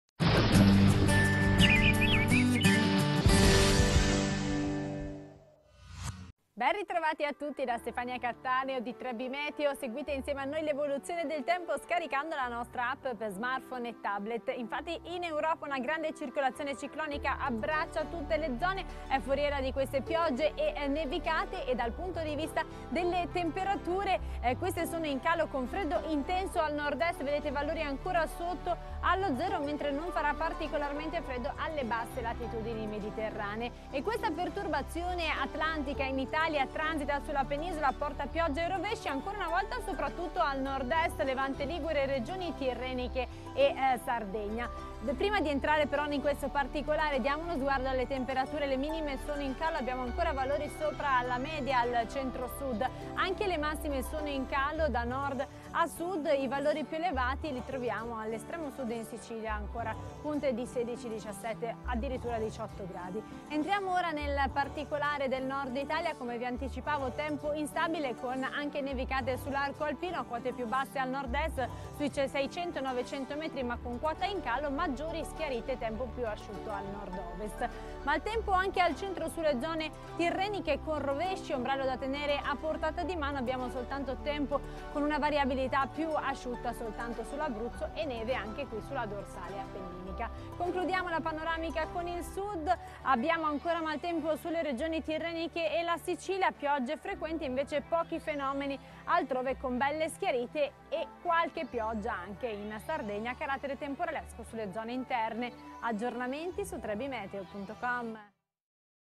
Previsioni meteo Video per domenica, 14 febbraio
Previsioni del tempo a cura di Stefania Cattaneo ☼ Vi ringraziamo per i mi piace e per le condivisioni ☼